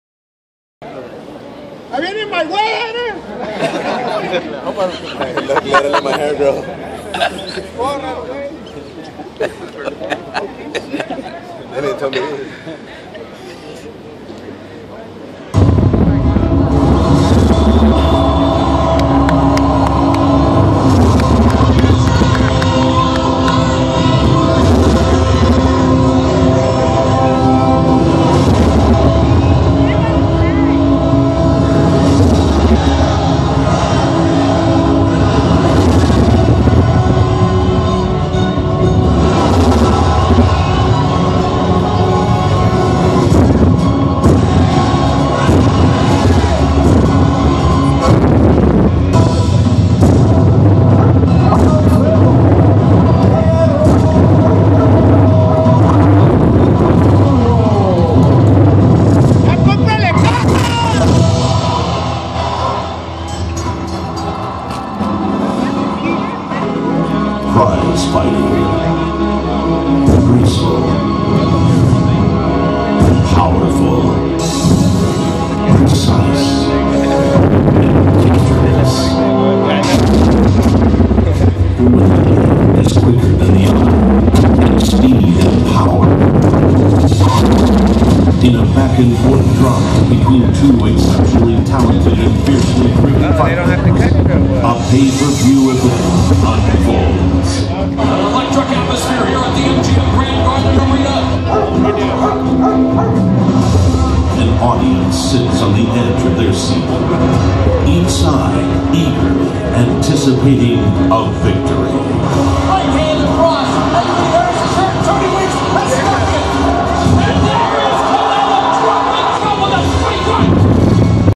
Alfredo Perro Angulo donating his hair
Perro Angulo donates his hair @ Canelo vs Angulo Press Conference in L.A.